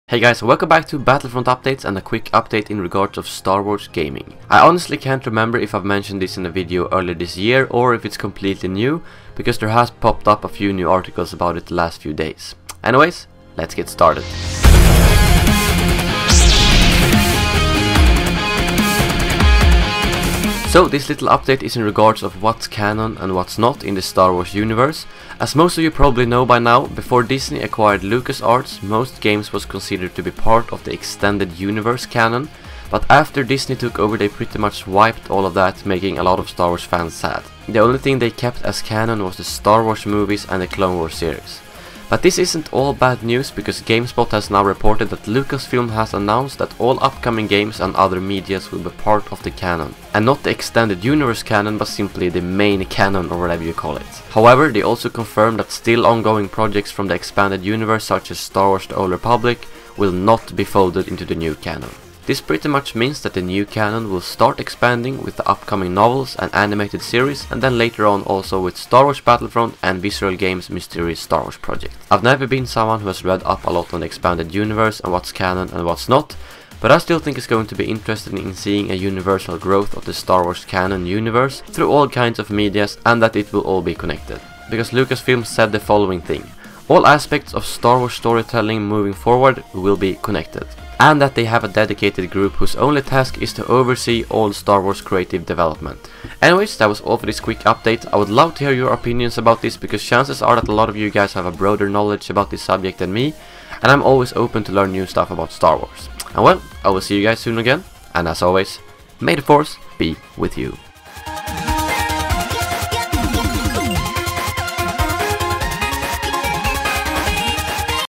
Star Wars Gaming News: All New Games Will Be Canon!
A quick little update on what's canon and what's not, nothing major but I know this is a subject a lot of people are interested in!

INTERESTING STUFF

SOURCES

MUSIC